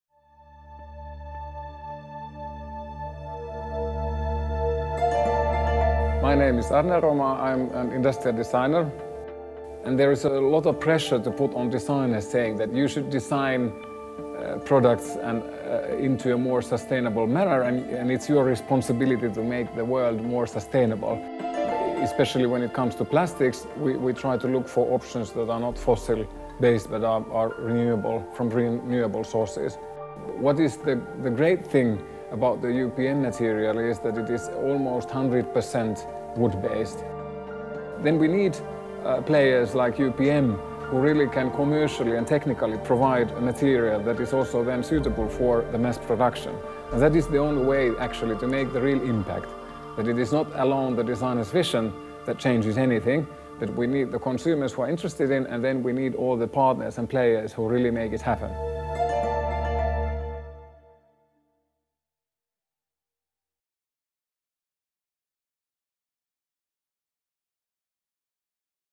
How to solve fossil plastics problem as a designer?
Sustainable design is one of the core values for Arni Aromaa, Creative Director and Partner of Pentagon Design, which is the agency behind Mysoda’s design. For the design team it was natural to look for alternatives for fossil-based material in this home carbonation market, which is a sustainable solution in itself. Pentagon Design had an ambitious target to create a timeless product that would take centre stage in the kitchen whilst blending seamlessly with the strong Finnish design heritage.